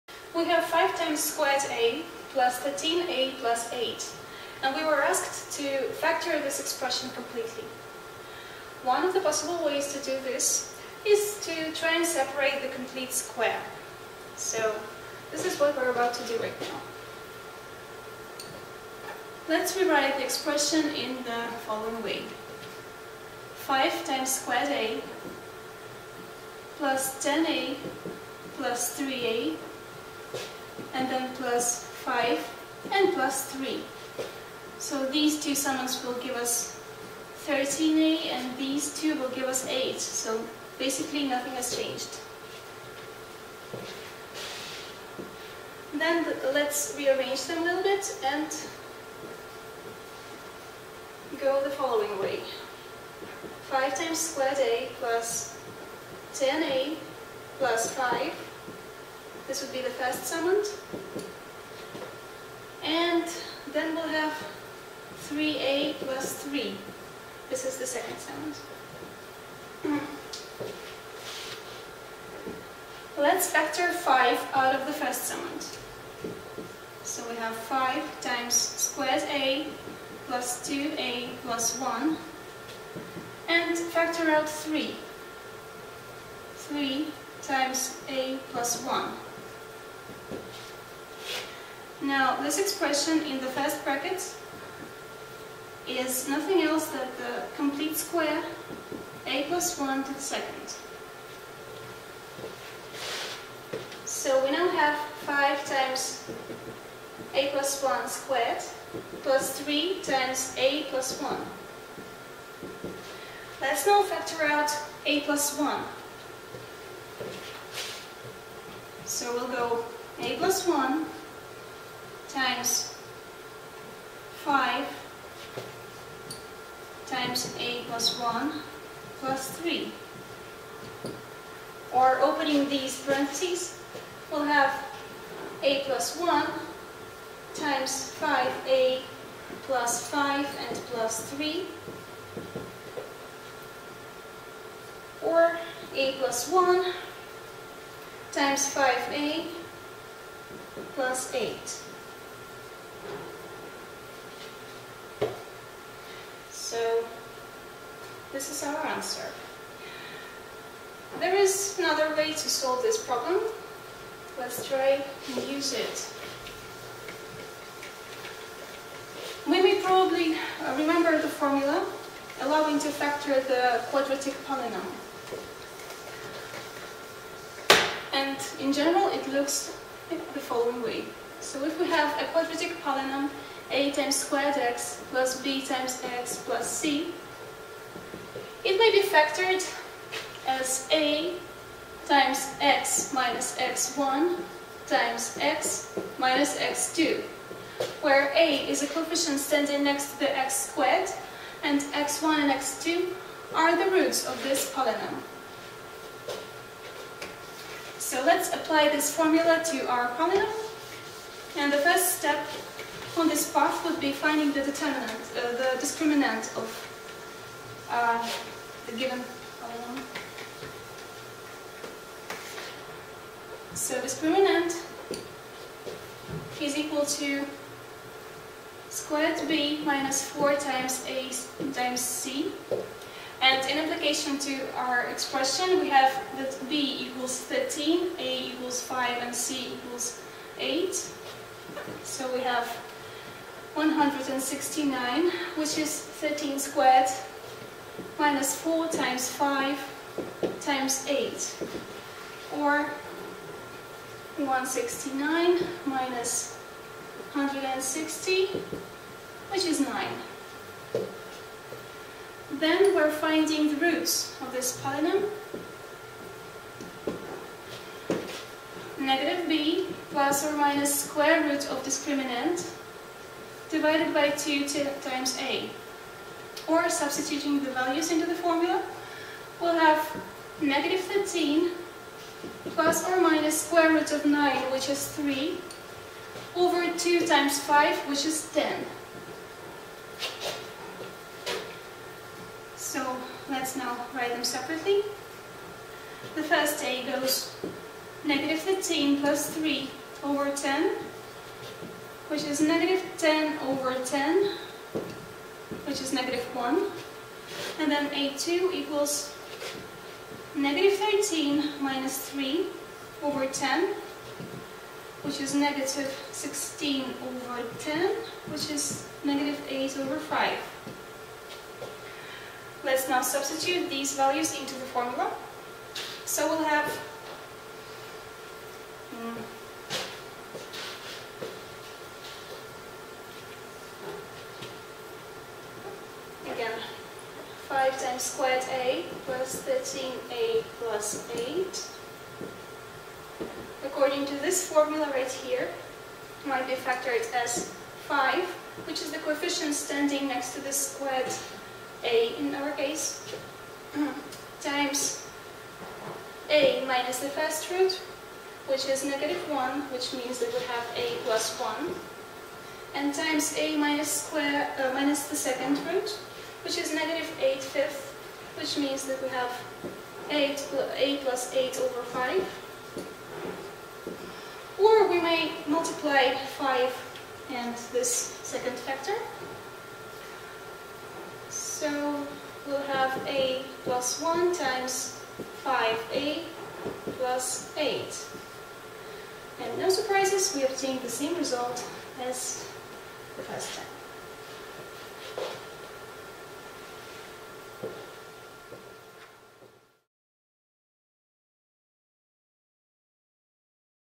Quadratic Factorization
This video shows how to factorize quadratic polynomials.
Initial problem is the following: it's required to factor completely quadratic polynomial 5a^2+13a+8.

Math Help: Elementary Algebra

Assignment Expert will help you to solve problems, answer you questions and provide detailed explanations.